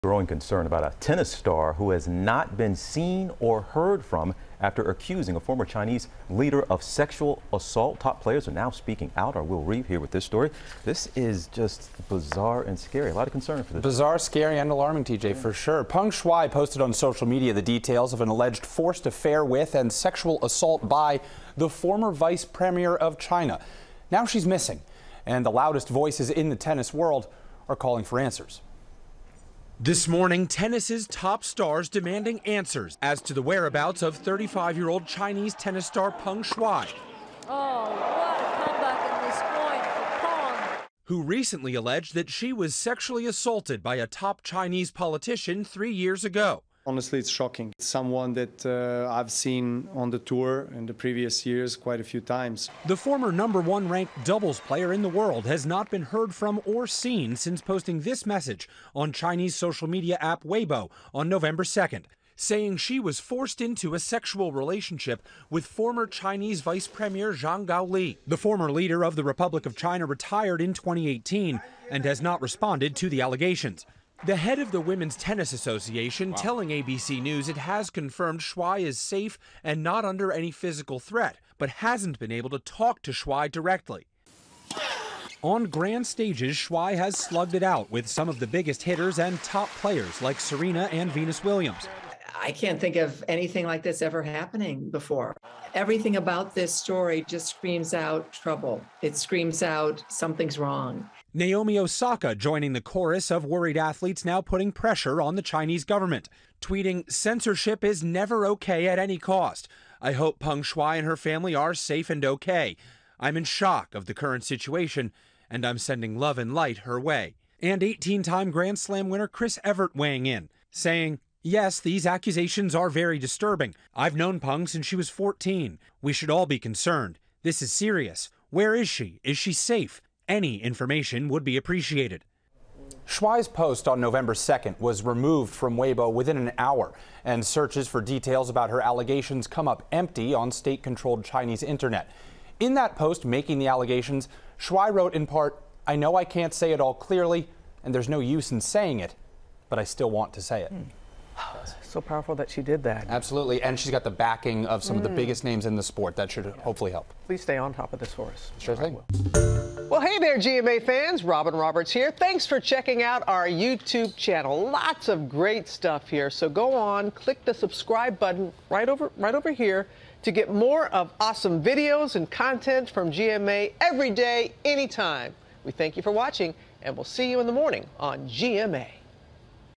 Tennis stars voice concern over missing Chinese player l GMA
Peng Shuai has not been seen or heard from since Nov. 2, after she accused a top Chinese politician of sexual assault.